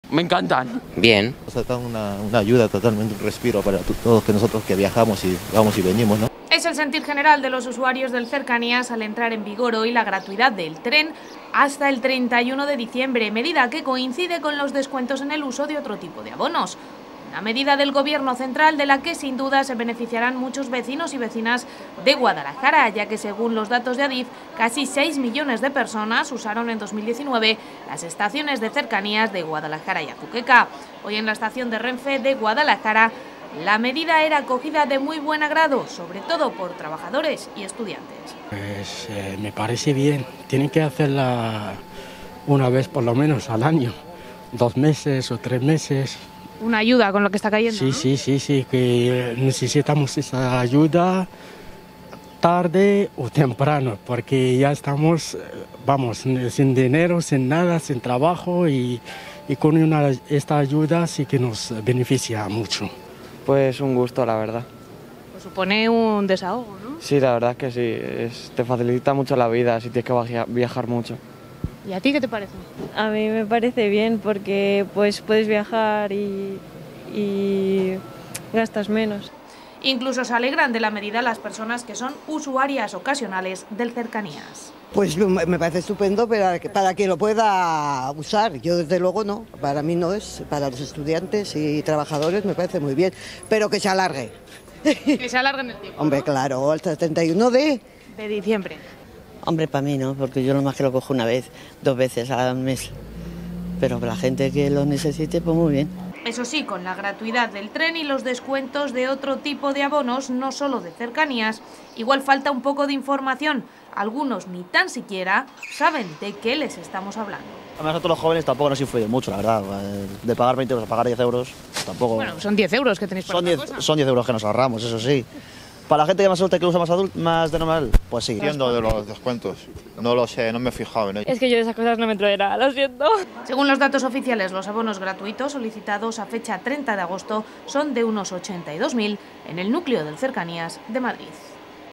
No todos saben que el cercanías es gratis
Se trata de una media del gobierno nacional que ha sido bien acogida pero que hay que solicitar debidamente y no todo el mundo se ha enterado.